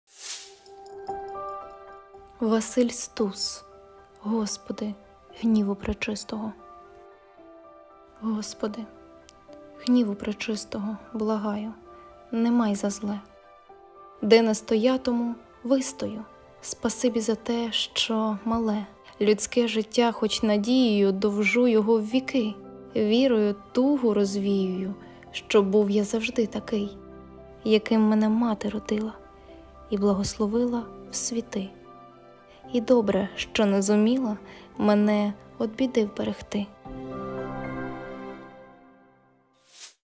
Василь Стус «Господи,гніву пречистого»
Декламування віршу Василя Стуса

Текст:
Господи, гніву пречистого
благаю — не май за зле.
Де не стоятиму — вистою.
Спасибі за те, що мале
людське життя, хоч надією
довжу його в віки.
Вірою тугу розвіюю,
щоб був я завжди такий,
яким мене мати родила
і благословила в світи.
І добре, що не зуміла
мене од біди вберегти.